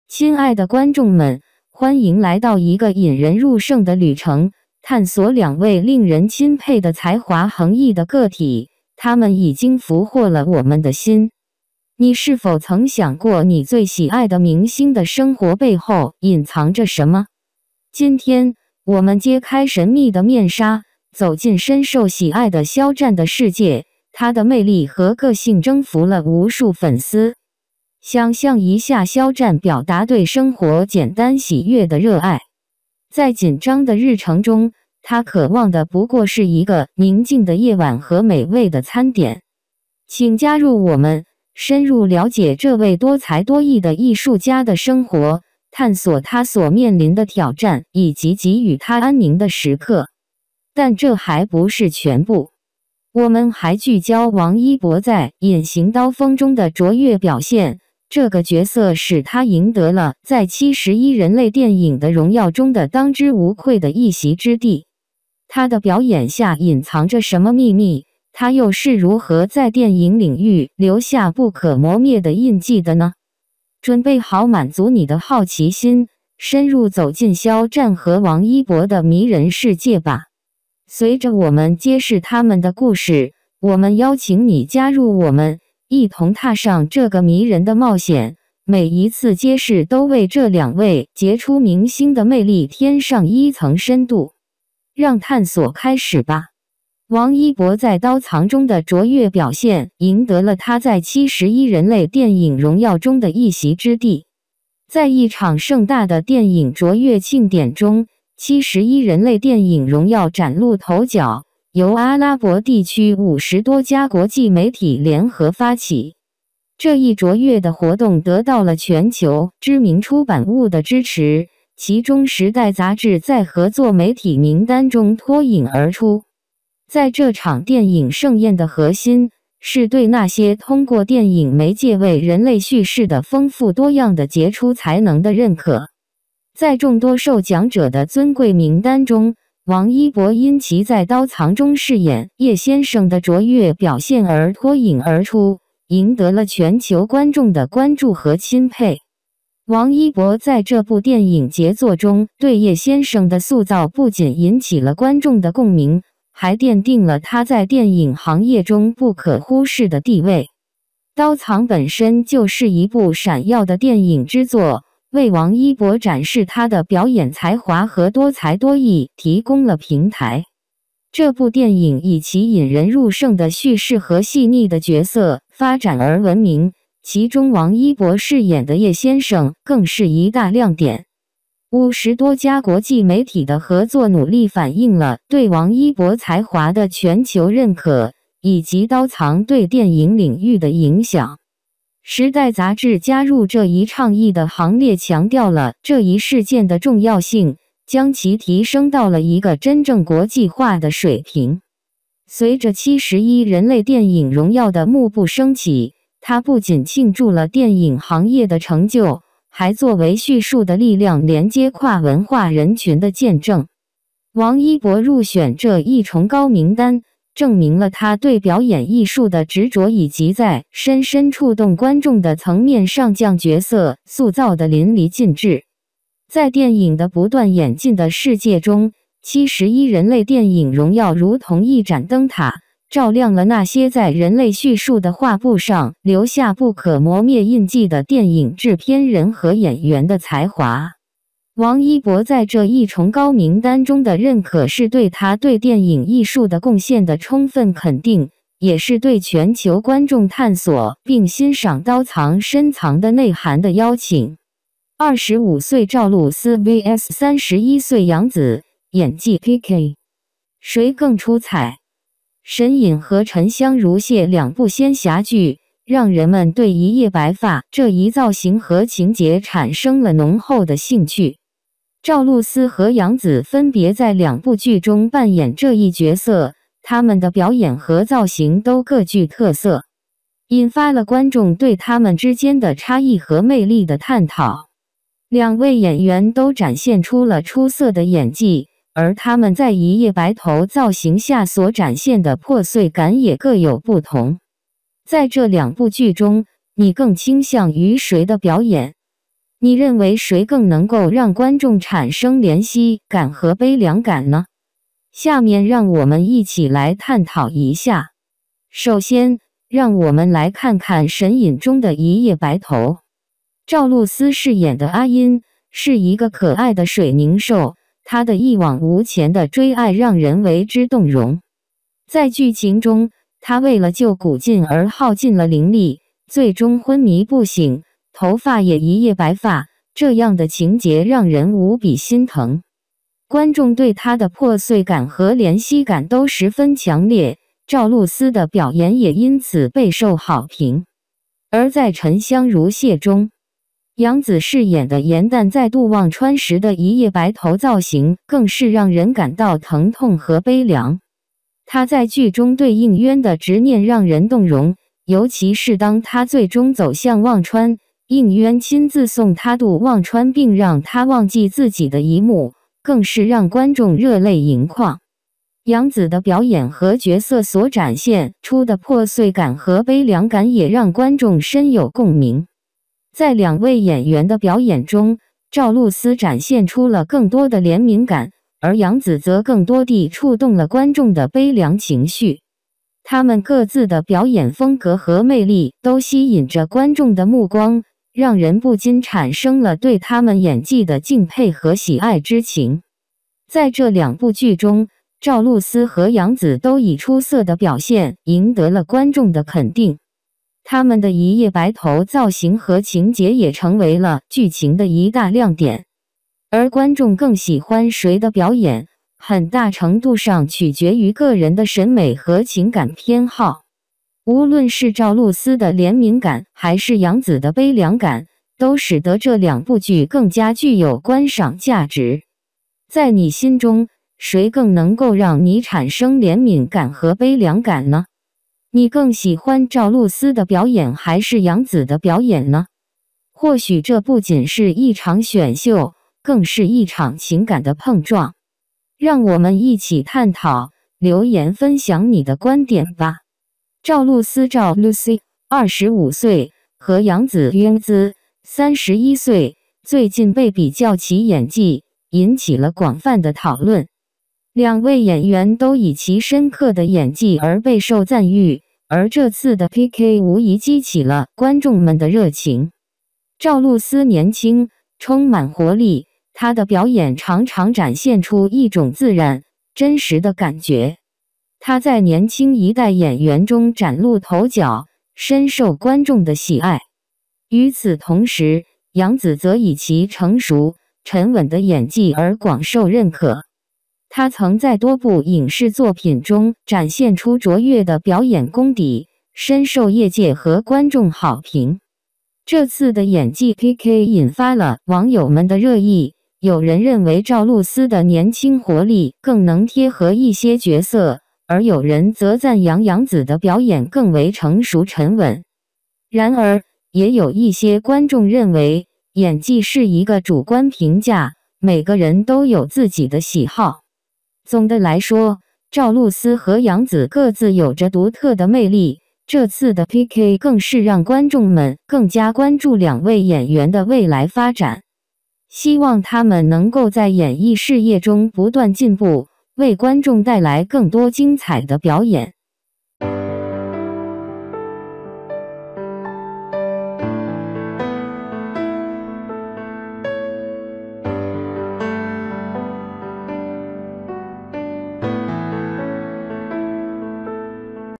肖战表示，有时间就只想睡个好觉，吃顿好饭，忙得不可开交。 王一博凭借《袖剑》的出色表现获得“71人类电影荣耀”
I love Xiao Zhan so much! Xiao Zhan said that if he has time, he just wants to have a good sleep and a good meal  he is so busy.  Wang Yibo's Stellar Performance in 'Hidden Blade' Earns Him a Spot in '71 Glory of Human Cinema